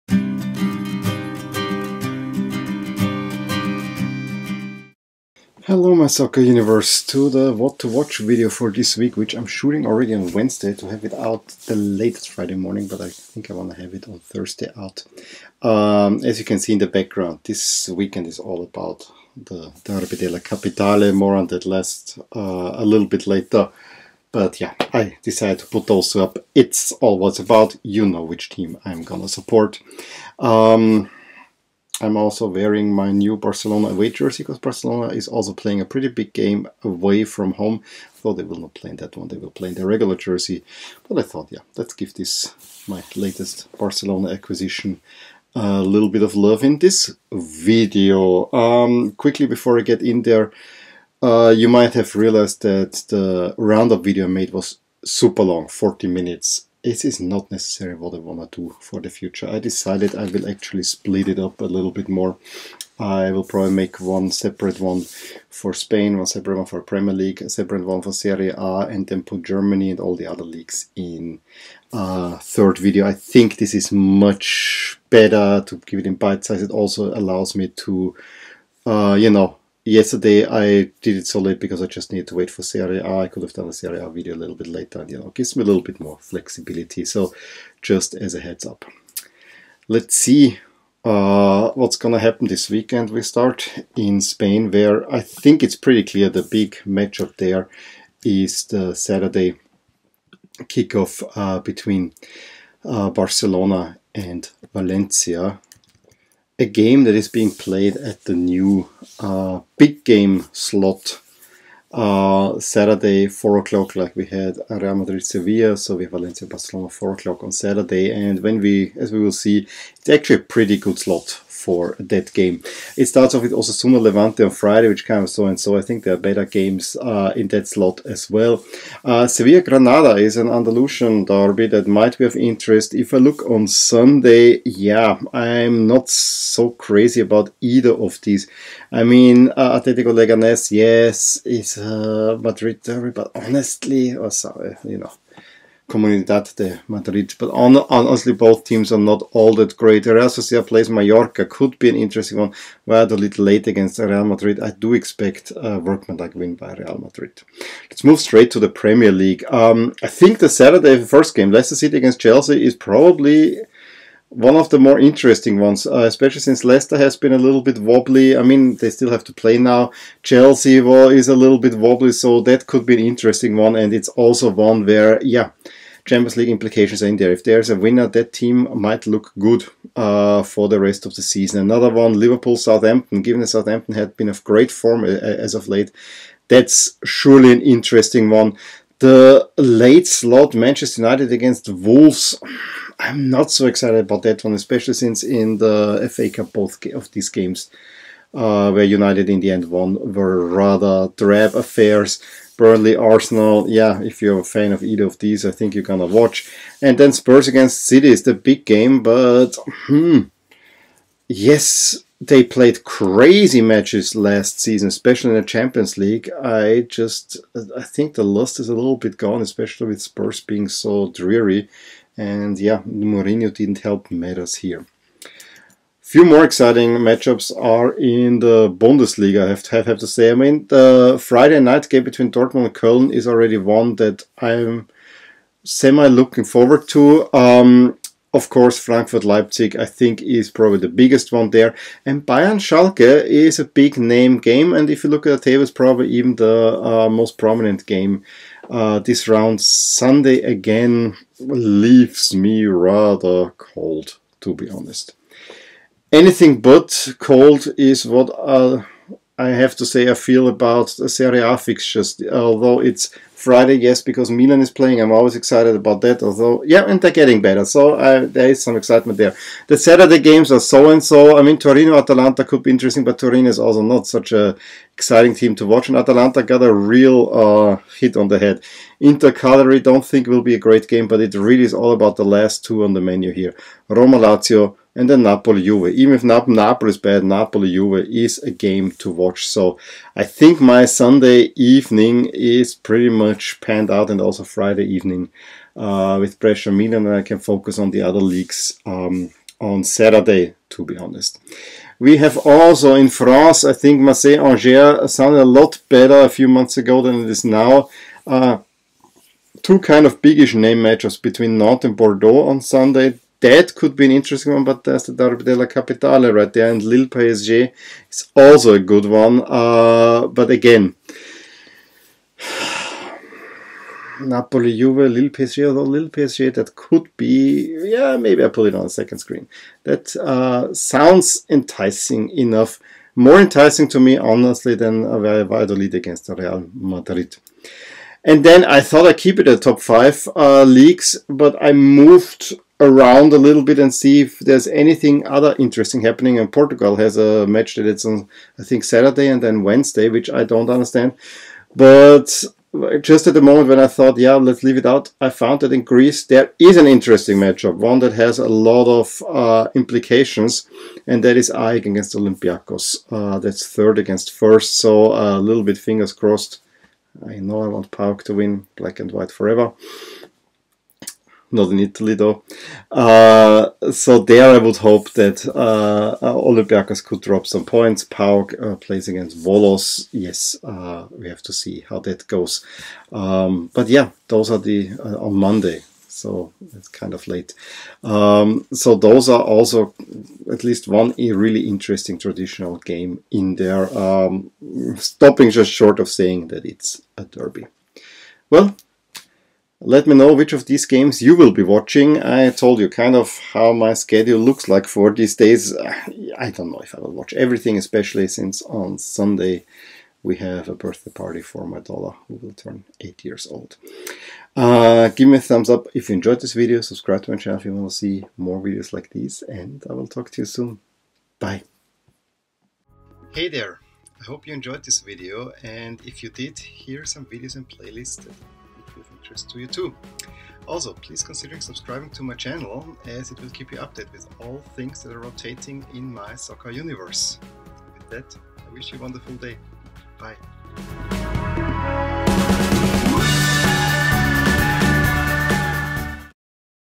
What to Watch: Jan 24-27, 2020 (Derby della Capitale and a Few Other Good Games)
The Roman derby surely is the biggest game this weekend, but also Valencia's clash with Barcelona, Spurs taking on City as well as Bayern facing Schalke surely are nice matchups. So an interesting weekend for sure!